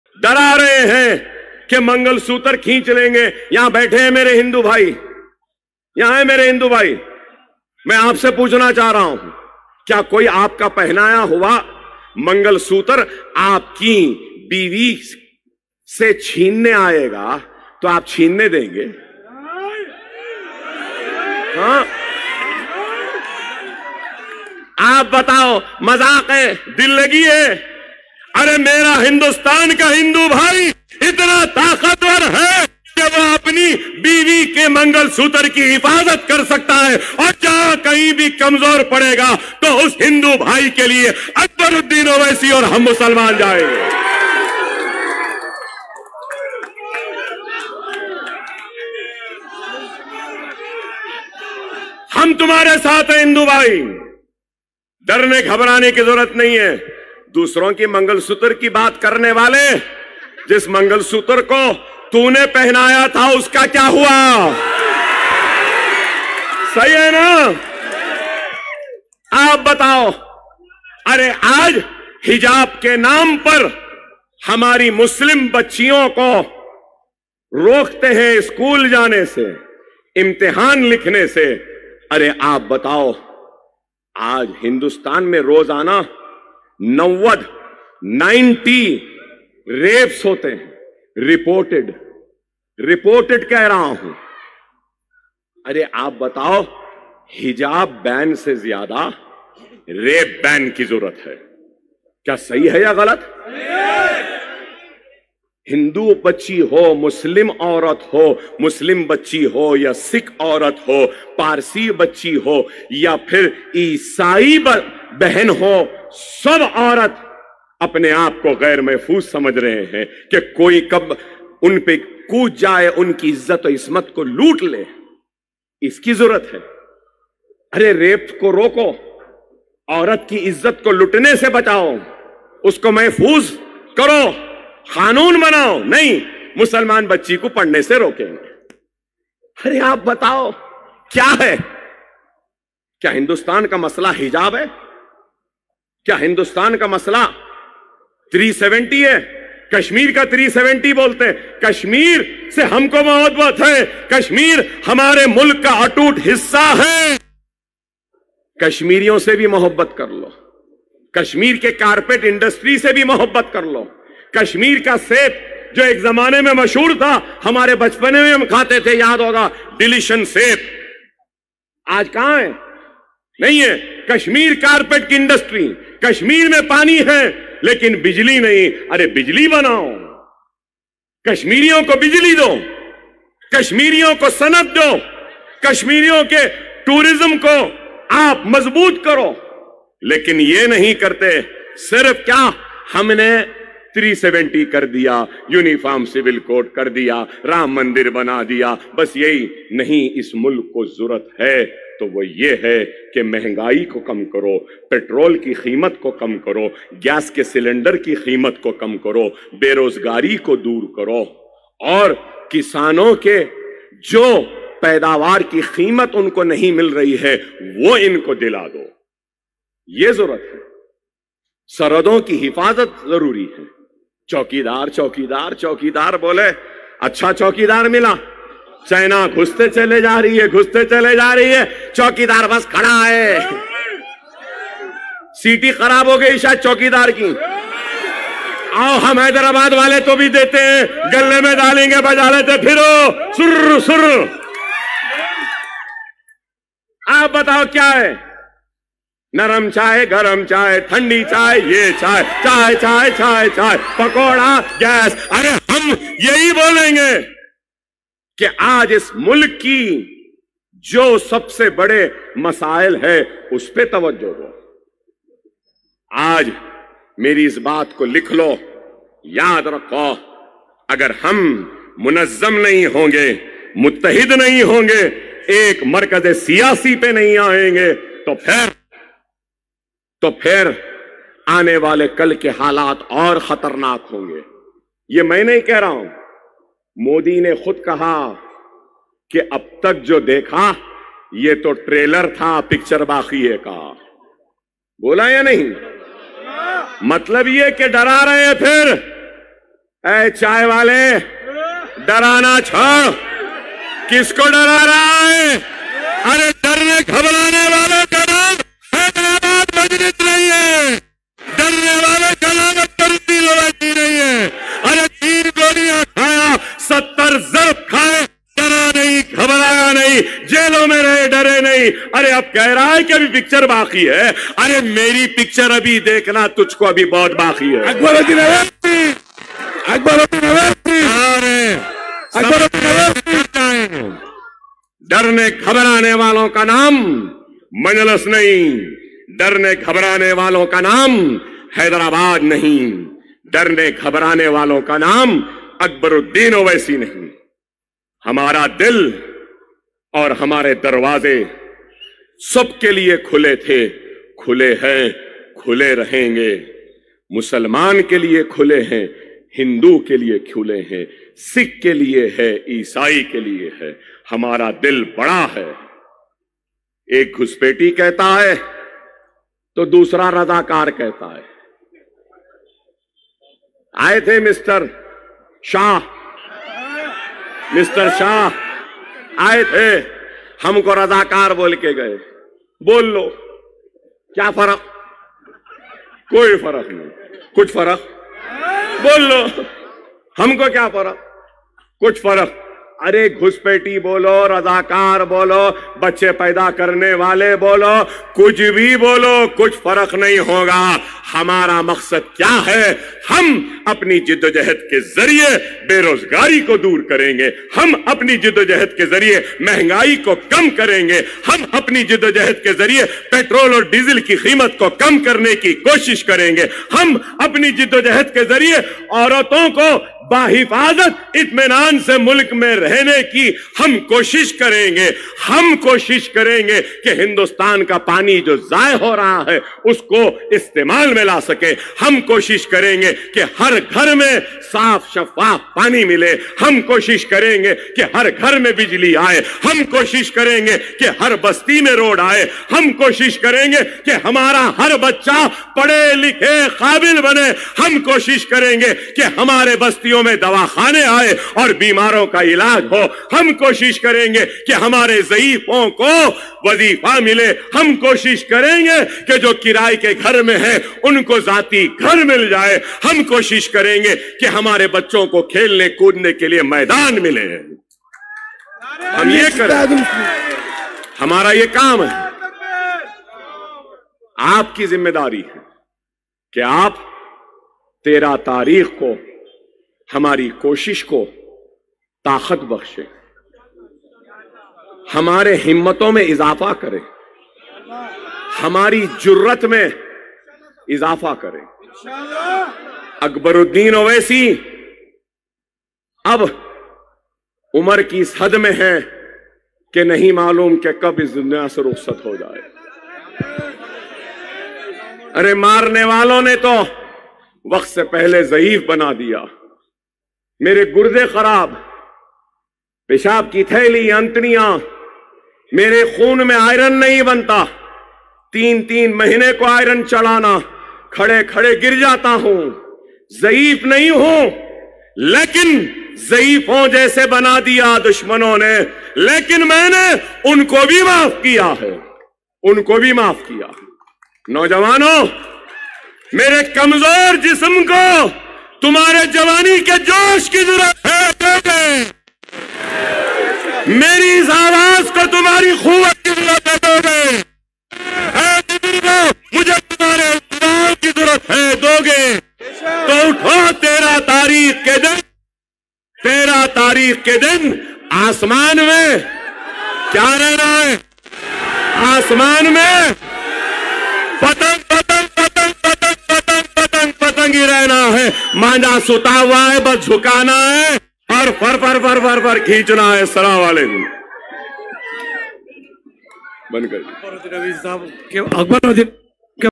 Akbaruddin Owaisi Hindu Bhai ko leke kya kaha dekho
Akbaruddin Owaisi hindu bhaiyon ko mangalsutr ko leke BJP walon ku kya kaha dekho

Yeh speech karwan Hyderabad ka hai 3rd may ka hai